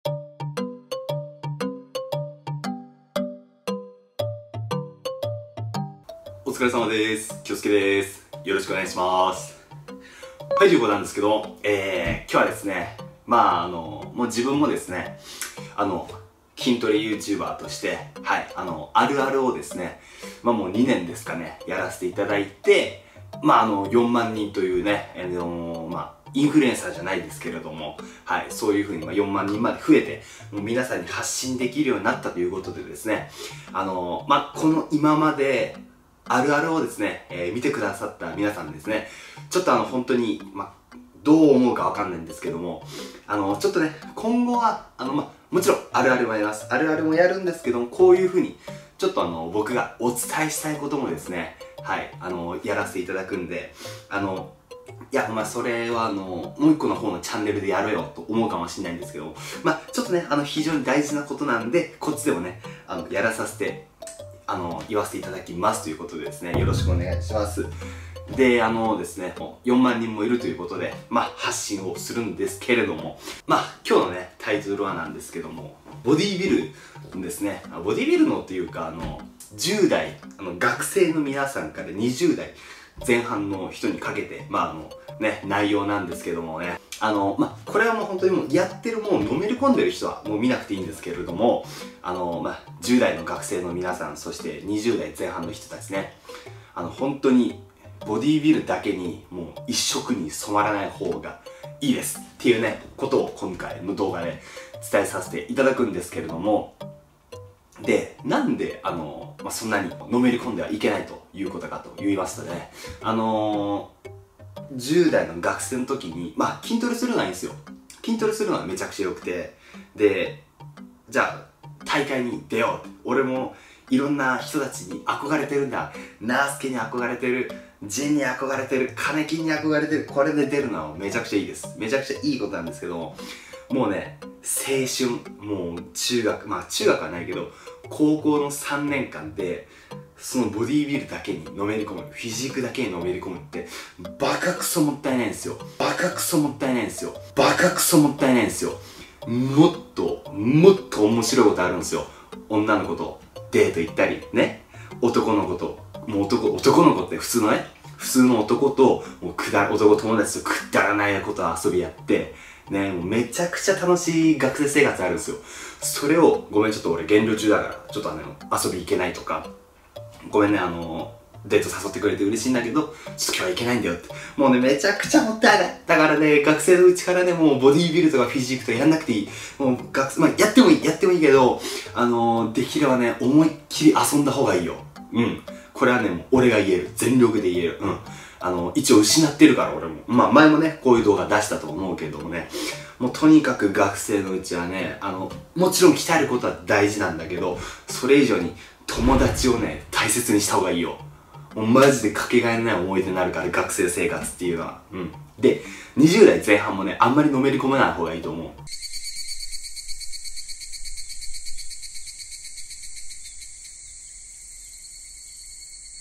【本気で伝えたいこと】学生や２０代前半の人たちはマジでボディビルやフィジークにのめり込まない方がいい。その理由を話すよ（学生編）
今も十分幸せダヒョ

2ndチャンネルはこちらになります！⁡
◆山崎恭介のやりたいことだらけ↓⁡
アパレルのサイトはこちらから！⁡